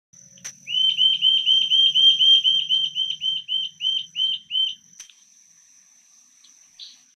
Rufous-tailed Jacamar Calling
Rufous-tailed Jacamar (Galbula ruficauda) calling. Recorded in Guatemala. Guide: Daniel Aldana